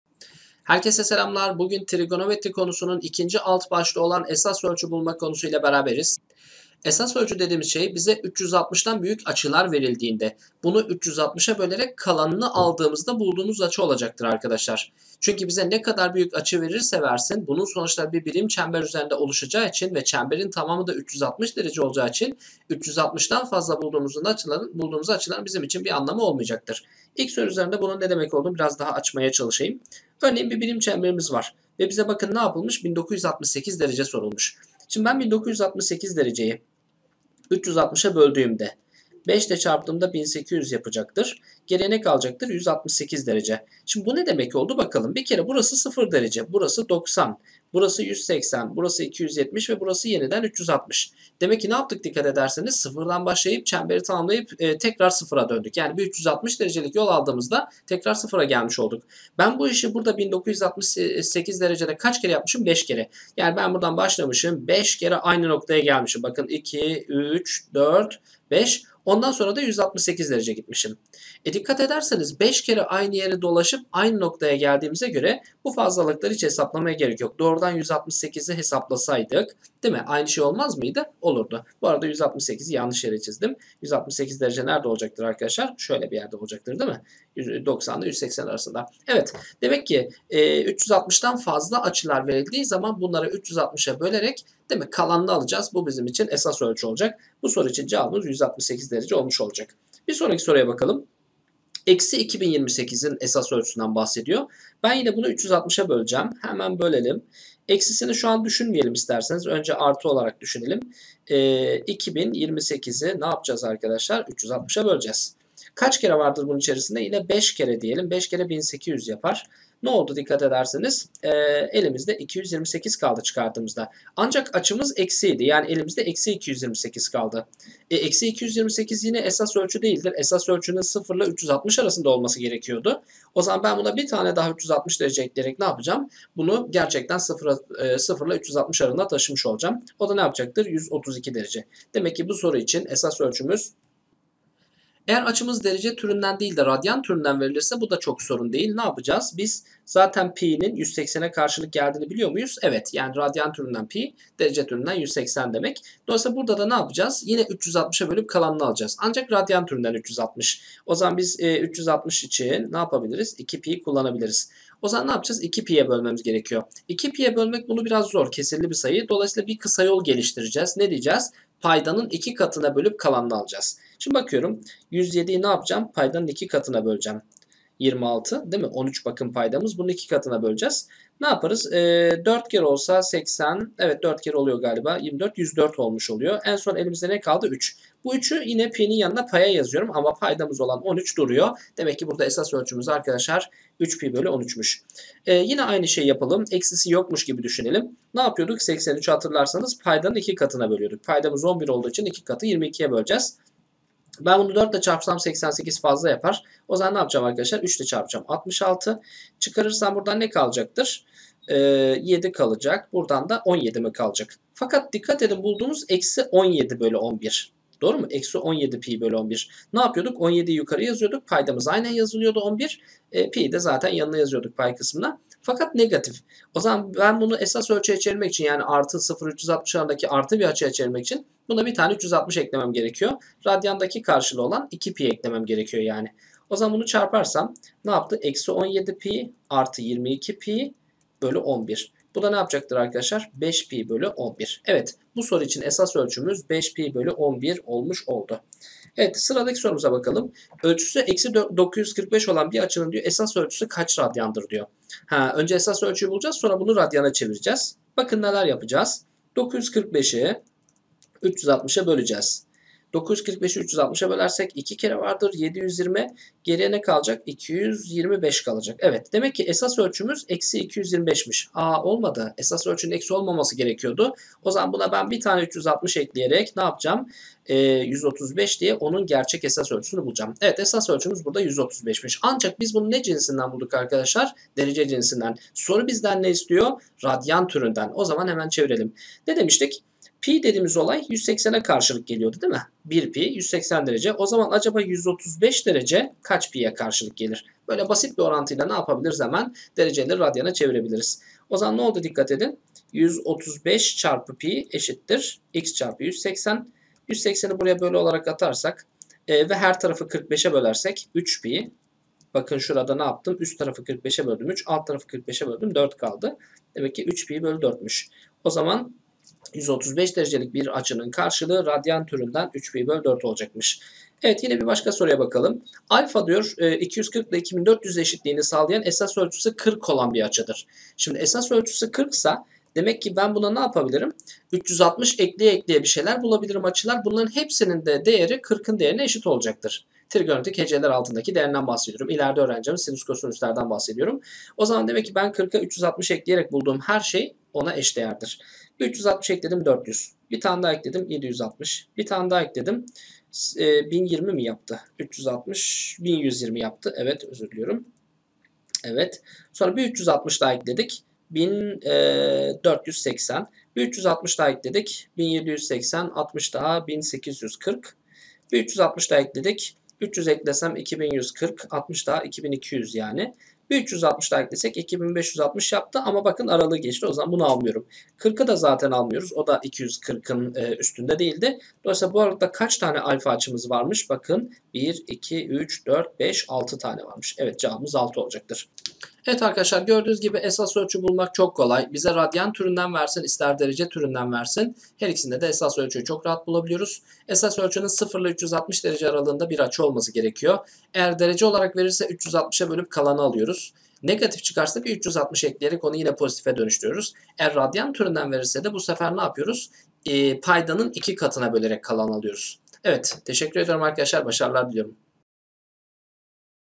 TRİGONOMETRİ ESAS ÖLÇÜ BULMA
Trigonometri konusunun ikinci alt başlığı olan "Esas ölçü bulma" videosuyla karşınızdayım.
Takipte kalın devamı yolda:)